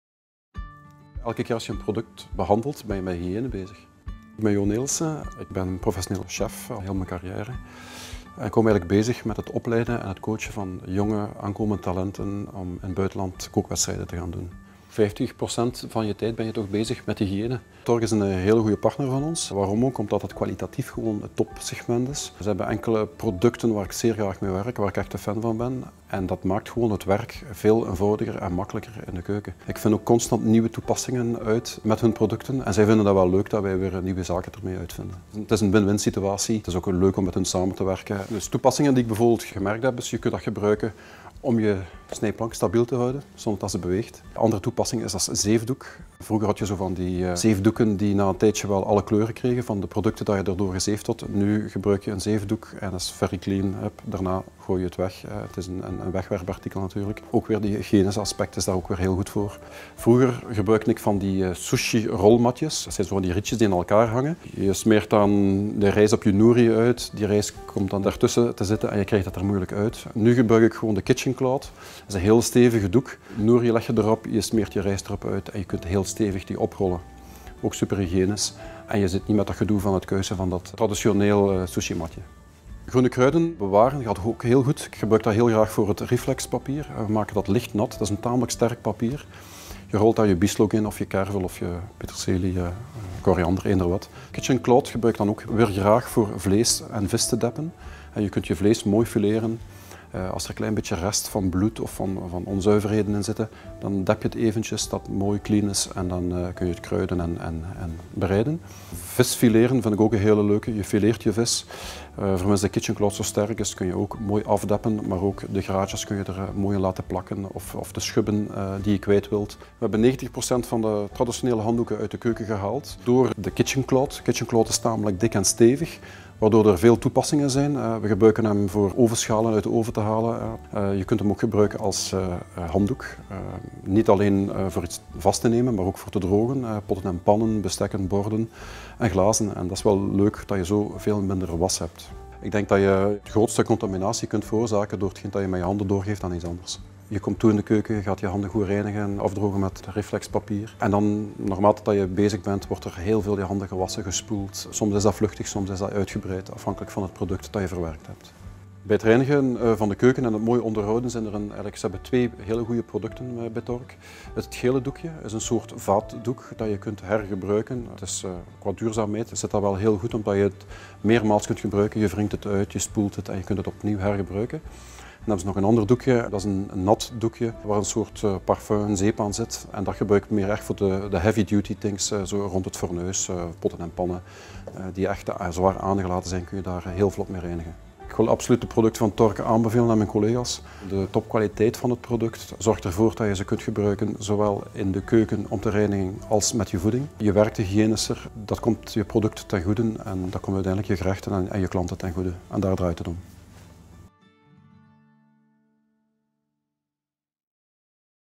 Jo Nelissen, grand chef cuisinier, présente les produits d'hygiène de cuisine Tork
L'hygiène dans les cuisines professionnelles est très importante. Quels produits Tork pouvez-vous utiliser pour quelle application ? Le grand chef Jo Nelissen de Horeca Vlaanderen, entraîneur de Junior National Culinary Team Belgium, vous montre à quoi vous pouvez les utiliser et vous donne également des conseils pour des applications supplémentaires utiles !